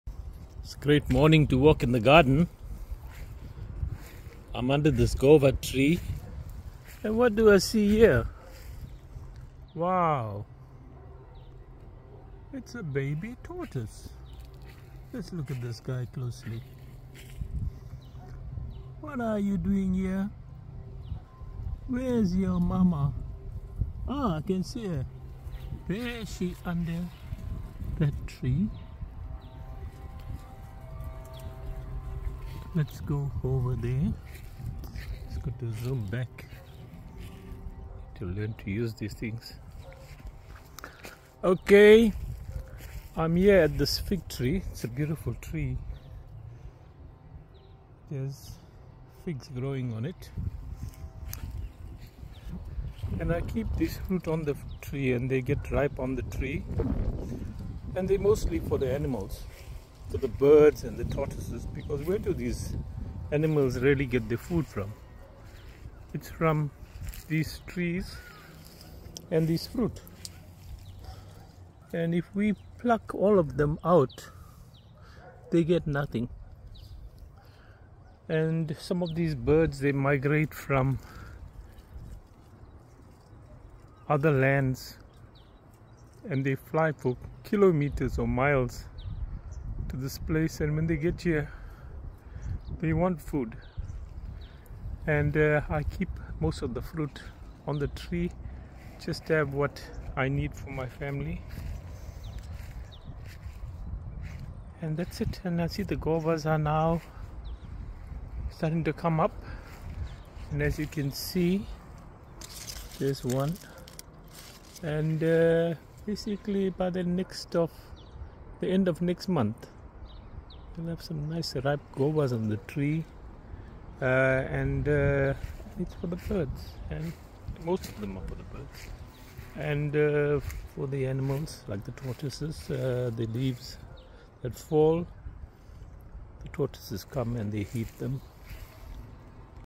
BABY TORTOISE VISITS ME on a beautiful morning
Its a rare occasion to see a mother tortoise with its baby. You will either see the baby or the mother. Today I was blessed enough to see them together.